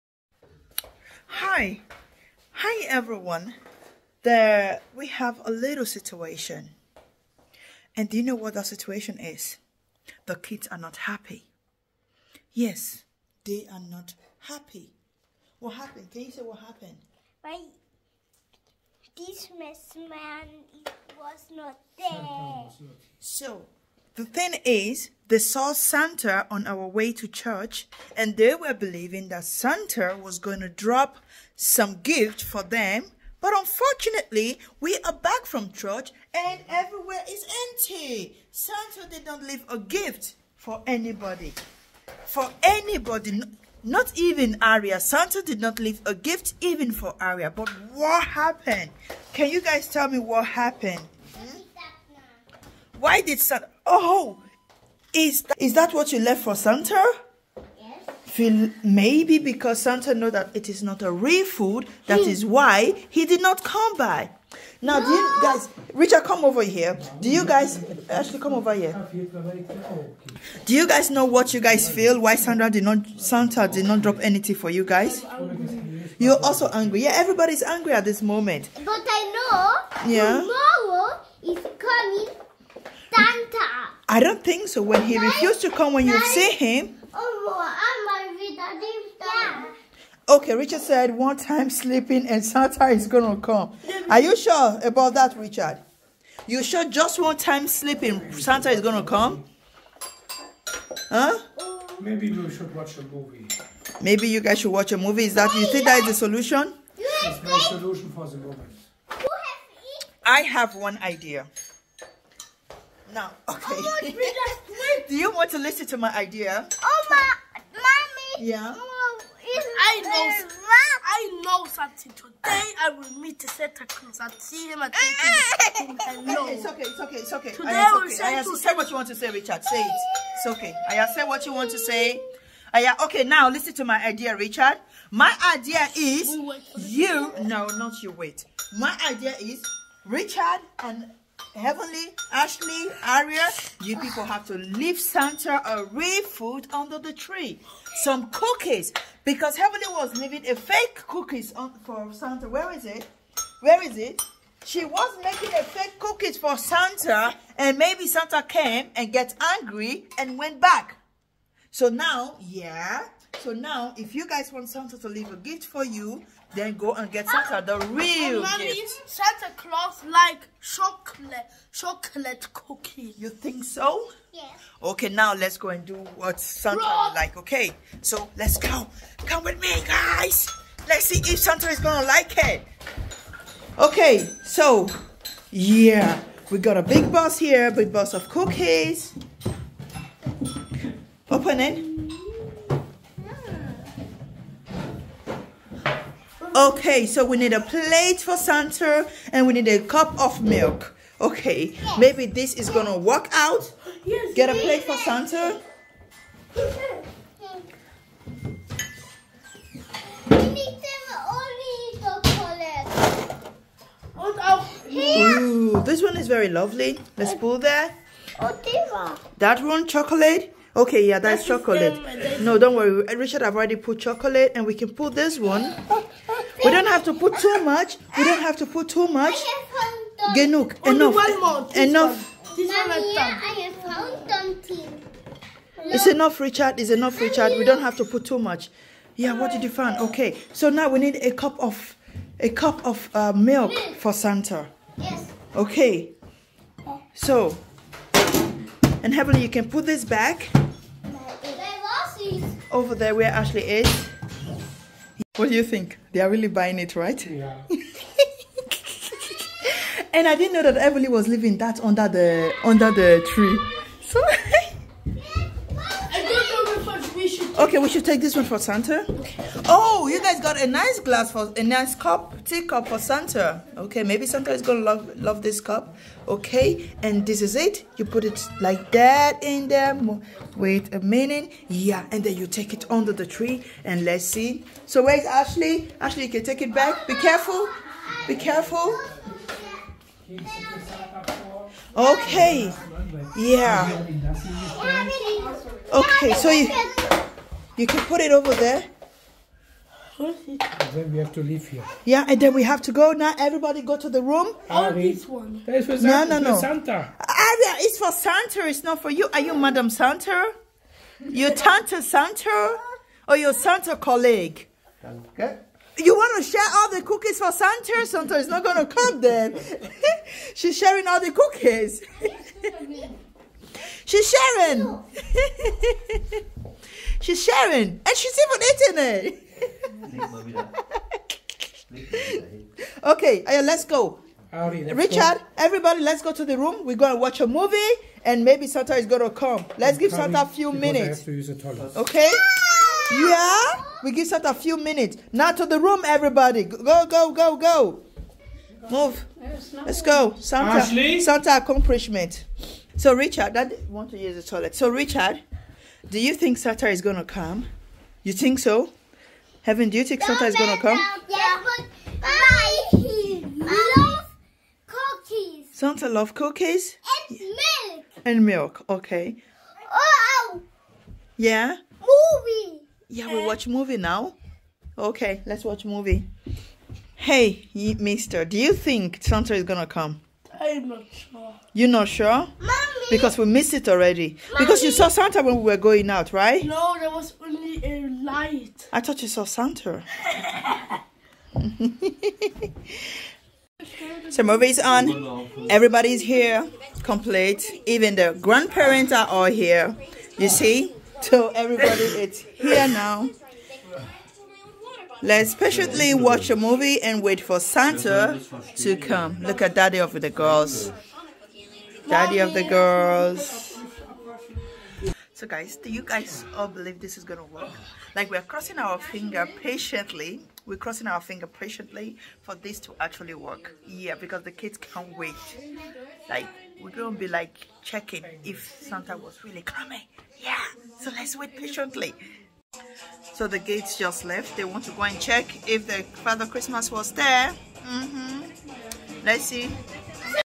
The kids aren't happy Santa didn't come by | Vlogmas
Merry Christmas to you all, as we go on a quest in solving this mystery why Santa Claus didn't drop gift for our kids we intend to carry you all along so we can solve this together 😊 😀 😉 ☺️